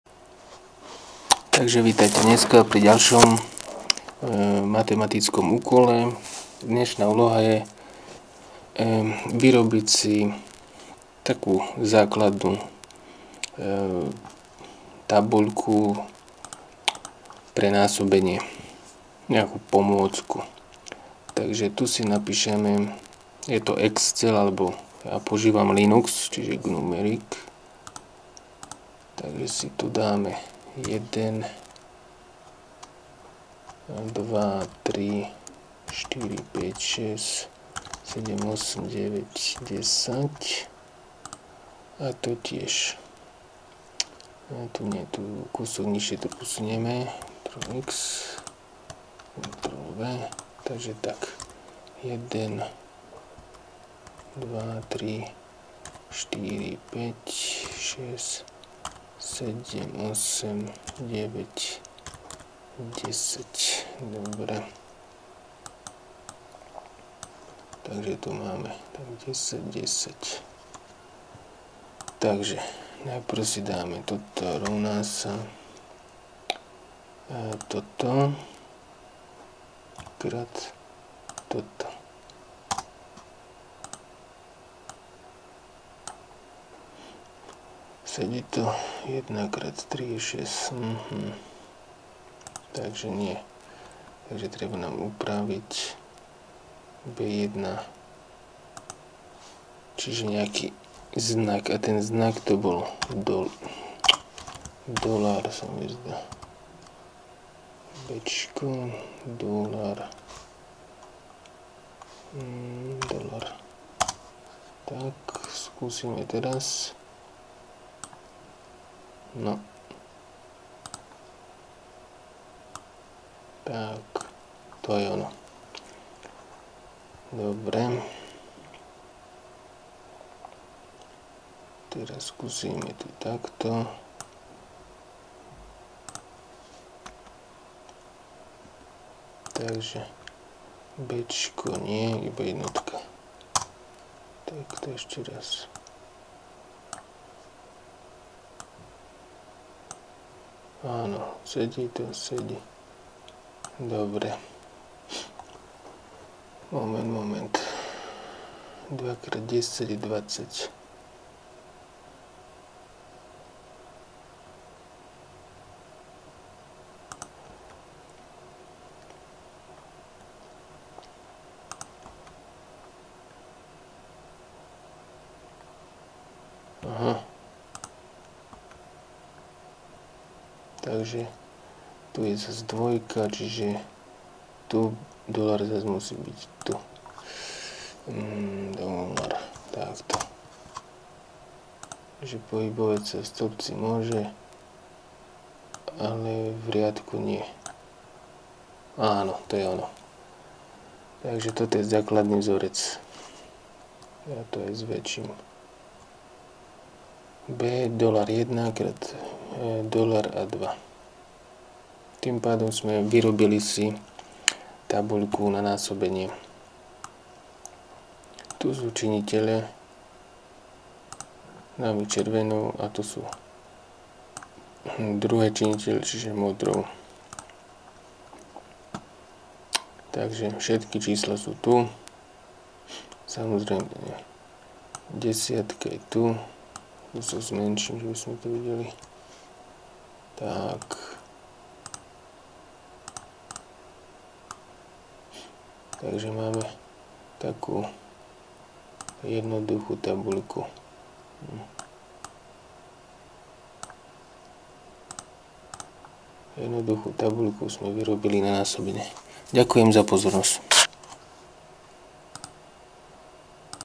Tabulka pre nasobenie do 5 minut
Slovensky.
Postup výroby základnej tabuľky násobenia.